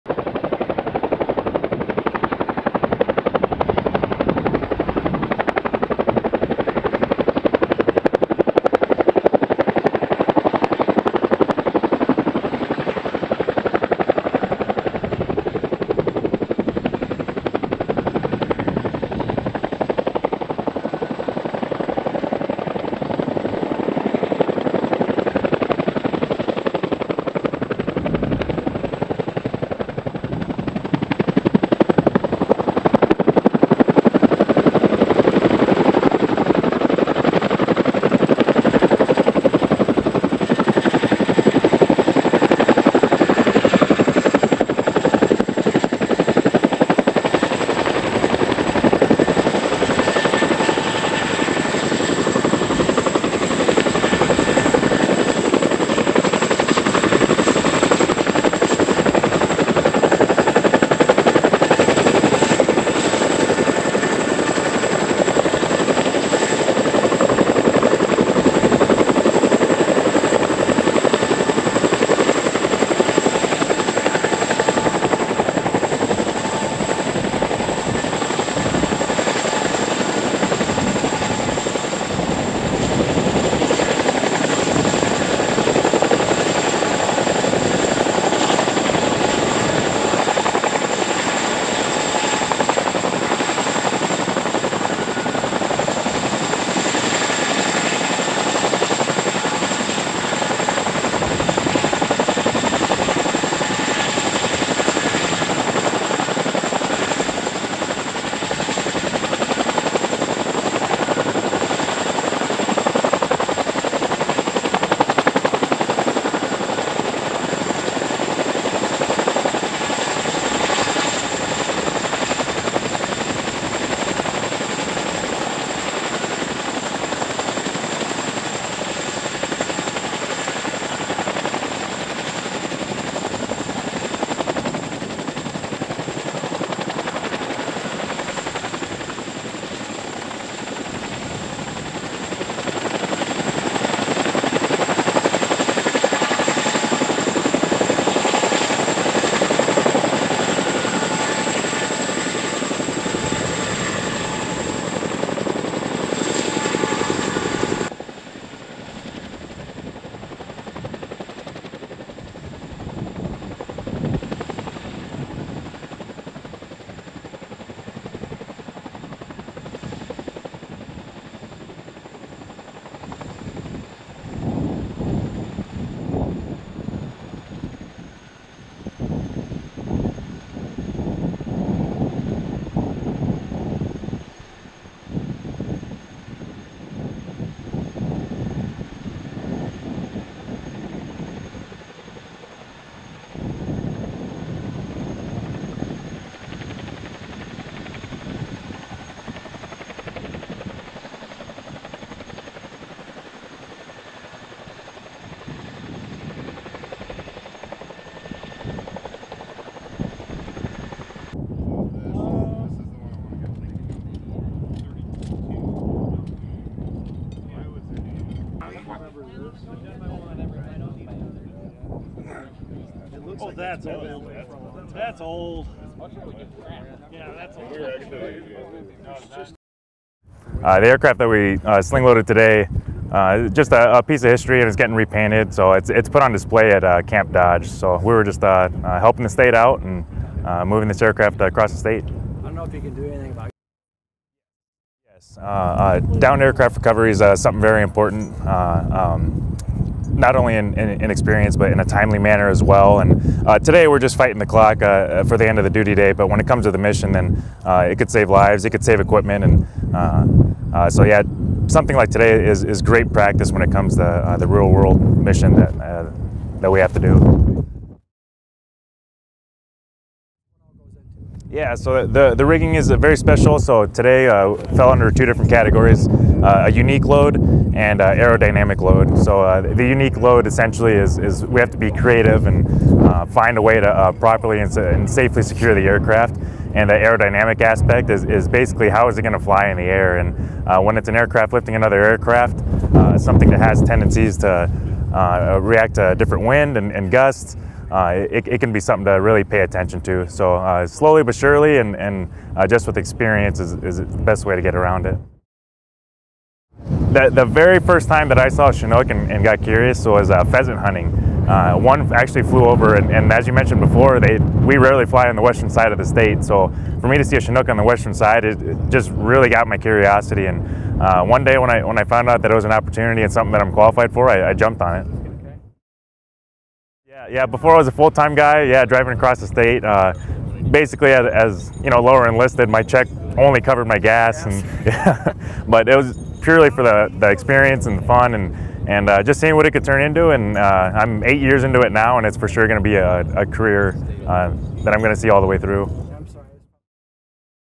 U.S. Army Chinook Sling Loads and Delivers an F-80 fighter jet
An Iowa Army National Guard helicopter from Davenport, Iowa delivers a historic F-80 fighter jet to the Air National Guard paint facility in Sioux City, Iowa where the jet will be repainted.

The 1950’s era aircraft was on display at Camp Dodge and has the markings of the 174th Fighter Interceptor Squadron that was the predecessor to the 185th Air Refueling Wing in Sioux City.

The Army CH-47 Chinook from B/171 Aviation Regiment is assigned to the Iowa National Guard. The Iowa Guard Soldiers delivered the jet to Sioux City where it made a gentle landing in front of the Air National Guard paint facility that is co-located with the 185th ARW.

The 1st generation fighter jet was assigned to the Iowa Air Guard during the 1950’s. The squadron was assigned the F-80 after the 174th returned to Sioux City following their activation in 1951.

During the flight to Sioux City this week the F-80 was in the capable hands of CH-47 Flight Engineer Staff Sgt. Jesse Ayala. Ayala said sling loading the aircraft to Sioux City allows them to practice important aircraft recovery methods.

After being painted, the aircraft will be placed back on static display at Camp Dodge were it will serve to educate visitors about the history of the Iowa National Guard.